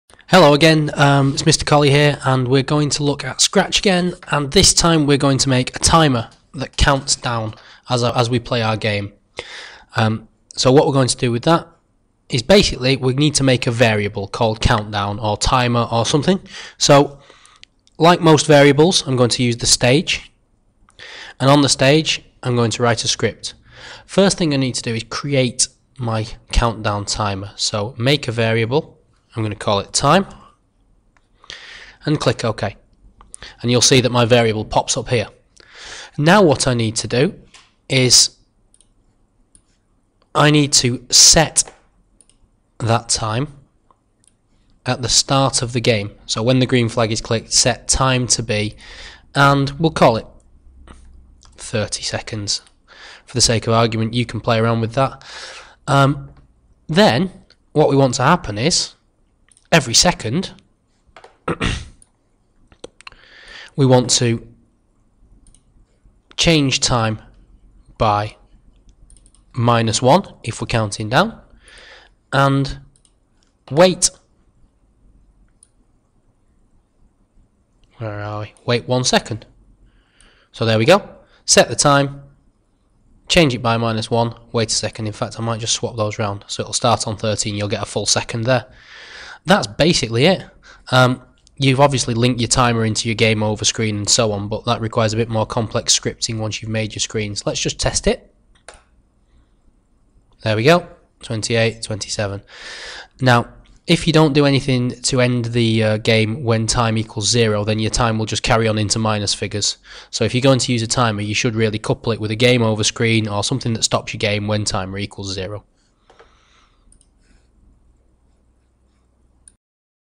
Timer.avi
Scratch tutorial showing how to add a timer to a game.